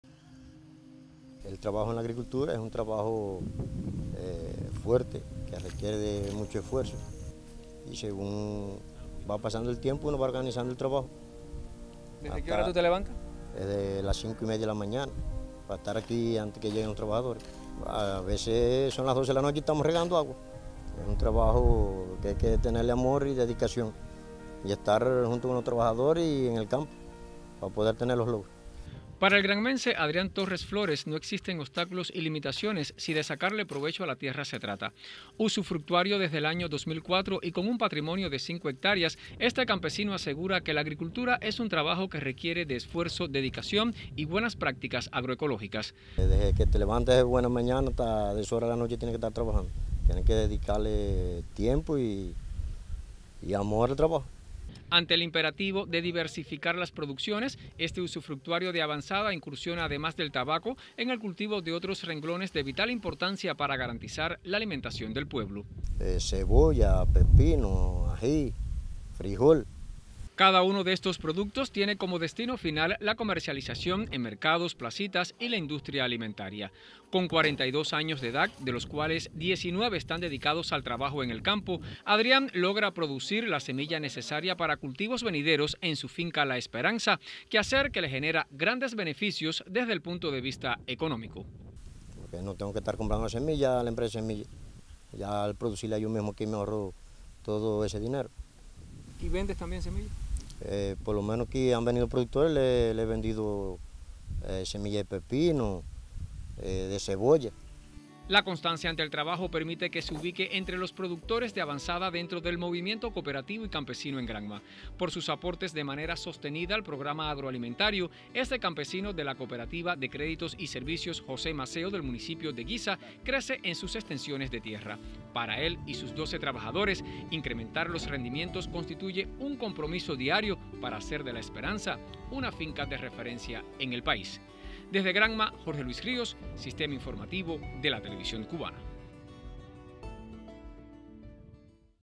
Adrián Torres figura entre los campesinos de avanzada en Granma
El campesino Adrián Torres Flores, figura entre los productores de avanzada en la provincia de Granma. Con cerca de dos décadas dedicado a las labores agrícolas, este usufructuario logra altos rendimientos no solo en el tabaco, su cultivo fundamental, sino también en otros renglones de vital importancia para la alimentación del pueblo.